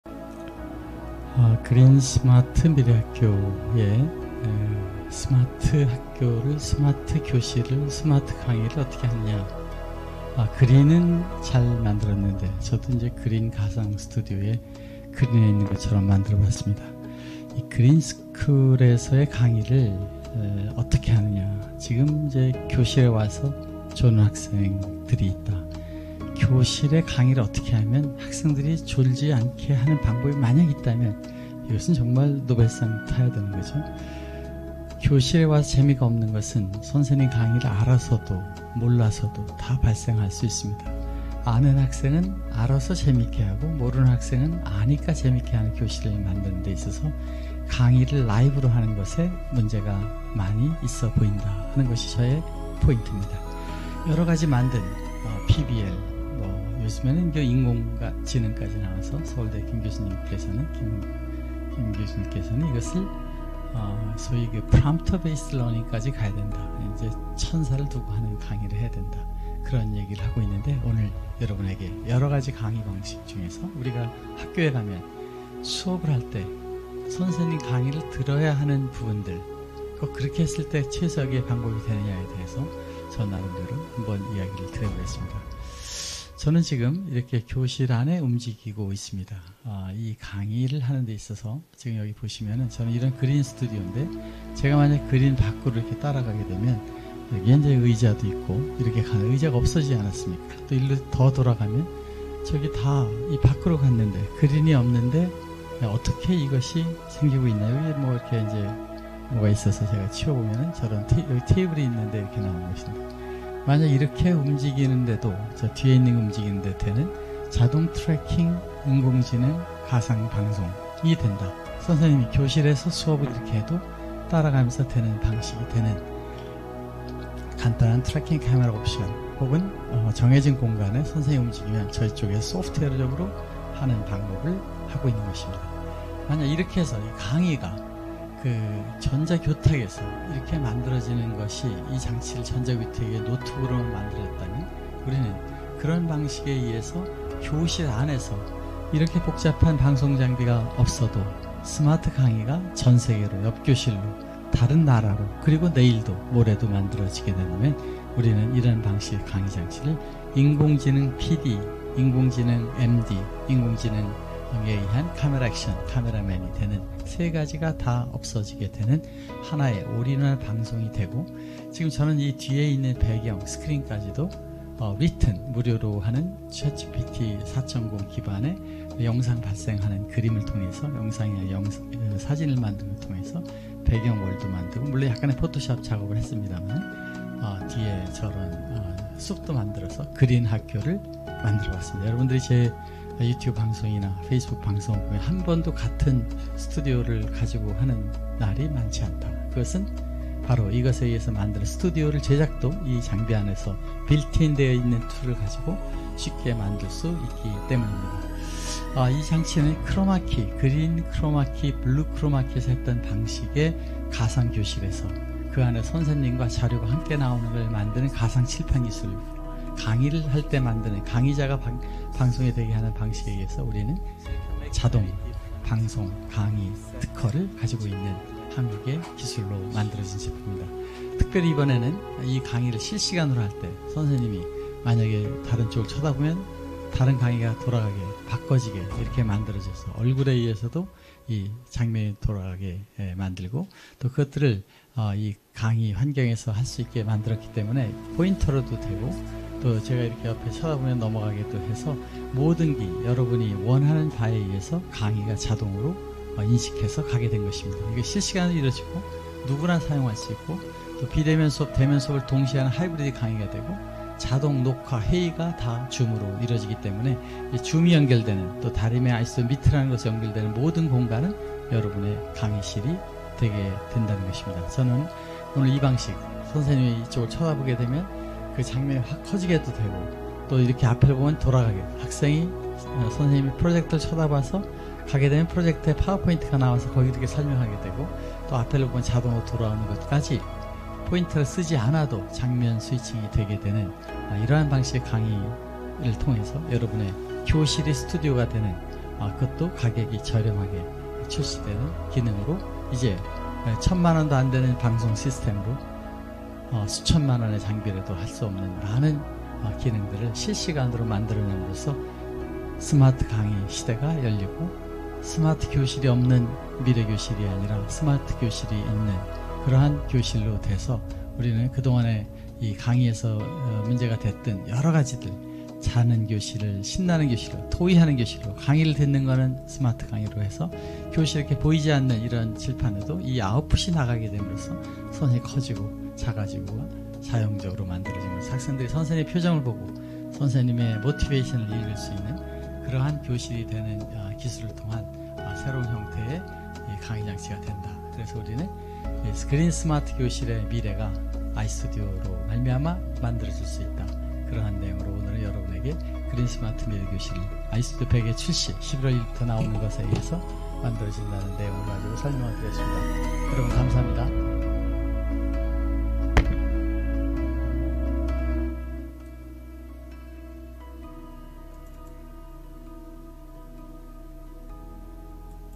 iStudio 100 AI 하이브리드 교실 - 자동 수업녹화 출시
iStudio 는 AI 강사 추적 및 AI PD 방식의 가상 강의 장치 특허 기술로  대면 수업은 더욱 몰입감 있게 , 비대면 수업은 대면 보다 더 집중되게 하는 자동 강사 트랙킹 시스템입니다.   기존의 수업 녹화 시스템에 SW 만 탑재해서도 첨단의 가상 강의실을 구성할수 있습니다.  그린 스마트 미래 학교에 그린 학교에 스마트 교실의 핵심 강의 교실, 첨단 전자 교탁시스템이 됩니다.